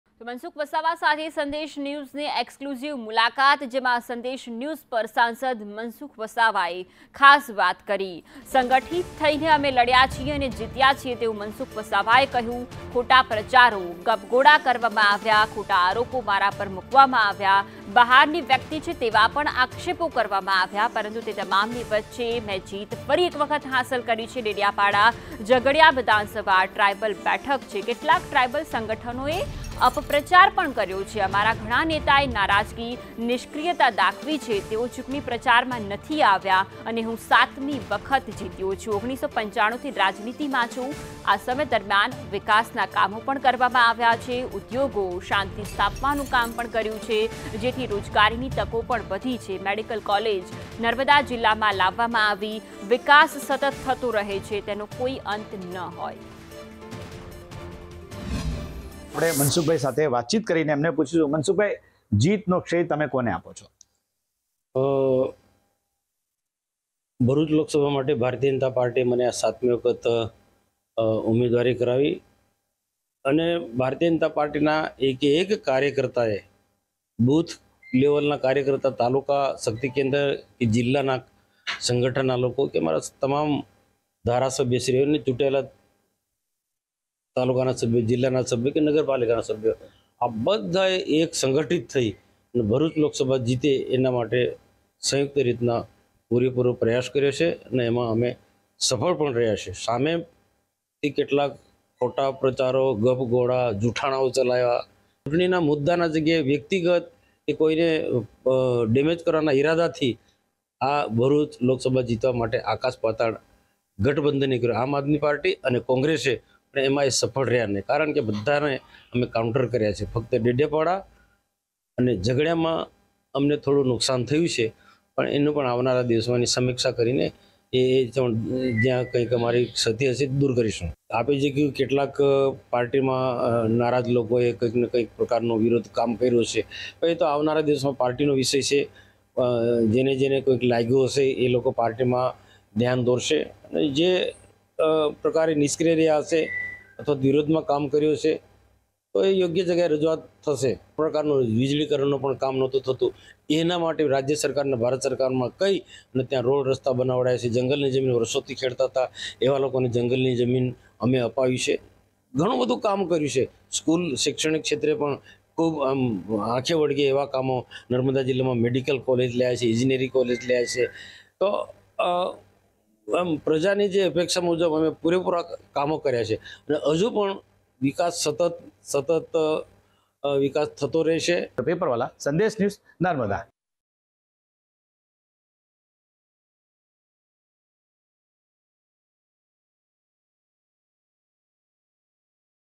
LokSabha Election Result 2024 | મનસુખ વસાવા સાથે સંદેશ ન્યૂઝની EXCLUSIVE મુલાકાત | BJP | Gujarat
મનસુખ વસાવા સાથે સંદેશ ન્યૂઝની EXCLUSIVE મુલાકાત
સંદેશ ન્યૂઝ પર સાંસદ મનસુખ વસાવાની ખાસ વાત
સંગઠિત થઈને અમે લડ્યા અને જીત્યાઃ મનસુખ વસાવા
ખોટા પ્રચારો, ગપગોળા કરાયાઃ મનસુખ વસાવા
ખોટા આરોપો મારા પર મુકાયાઃ મનસુખ વસાવા
બહારના વ્યક્તિ છે તેવા આક્ષેપો કરાયાઃ મનસુખ વસાવા
‘ડેડિયાપાડા, ઝઘડિયા વિધાનસભા ટ્રાયબલ બેઠક છે’
કેટલાક ટ્રાયબલ સંગઠનોએ અપપ્રચાર કર્યોઃ મનસુખ વસાવા
‘અમારા ઘણા નેતાએ નારાજગી, નિષ્ક્રિયતા દાખવી’
તેઓ ચૂંટણી પ્રચારમાં ના આવ્યાઃ મનસુખ વસાવા
‘હું 7મી વખત જીત્યો છું, 1995થી રાજનીતિમાં છું’
આ સમય દરમિયાન વિકાસના કામો કર્યાઃ મનસુખ વસાવા
ઉદ્યોગો, શાંતિ સ્થાપવાનું કામ કર્યું : મનસુખ વસાવા
જેથી રોજગારીની તકો વધીઃ મનસુખ વસાવા
મેડિકલ કોલેજ નર્મદા જિલ્લામાં લાવ્યાઃ મનસુખ વસાવા
‘વિકાસ સતત થતો રહે છે તેનો કોઈ અંત ન હોય’